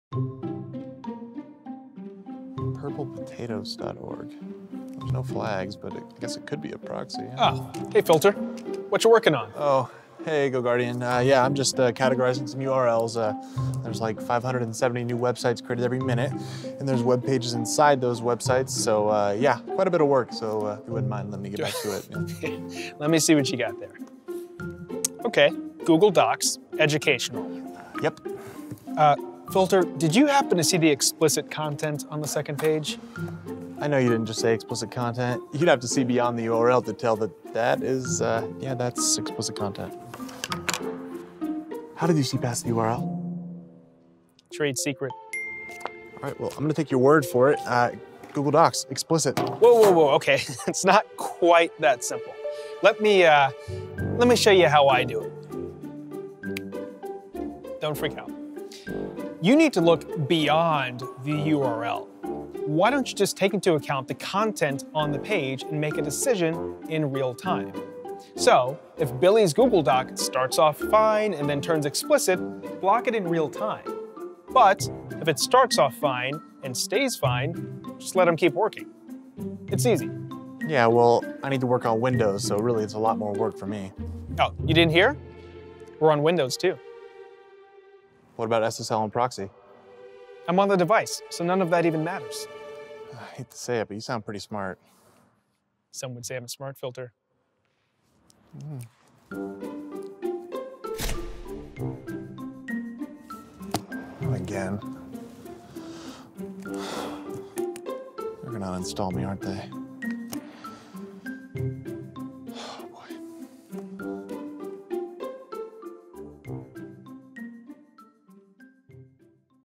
Introducing Smart Filtering | GoGuardian
Smart Filtering™ is up to 100x more effective than traditional URL filters.

Smart Filtering helps you create safe and effective learning environments. At GoGuardian we're passionate about keeping students safe and on task. Our award-winning team of engineers and data scientists have built the next-generation of filtering, and the results are astounding. Only GoGuardian technology can help determine the meaning and intent of student browsing activity, making more effective decisions about what to allow, block, or notify.

Unlike other solutions, Smart Filtering™ uses GoGuardian's proprietary neural network to continuously understand context within the browser. GoGuardian gives admins full control over what we call Smart Rules, or customizable actions and insights.

Dir. Rob Lynn